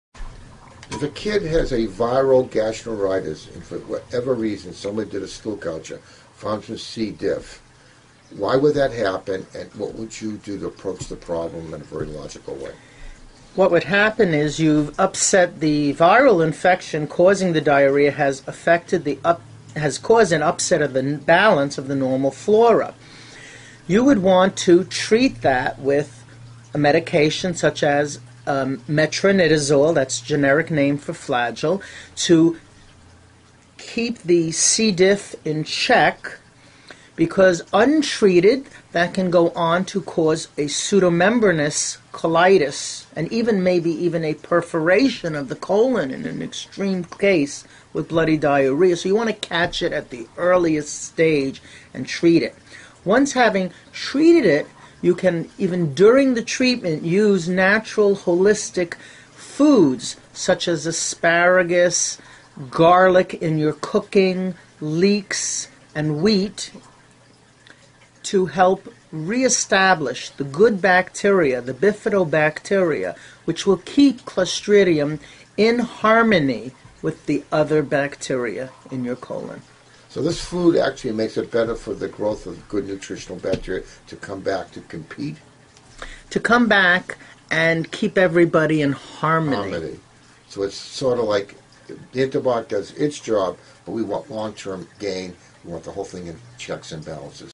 Can Clostridium difficile follow other infections
Martin D. Fried MD
Fellowships: Hospital For Sick Kids, Toronto - Pediatric Nutrtion & Babies Hospital,
Columbia Presbyterian Med Ctr, NY - Pediatric Gastroenterology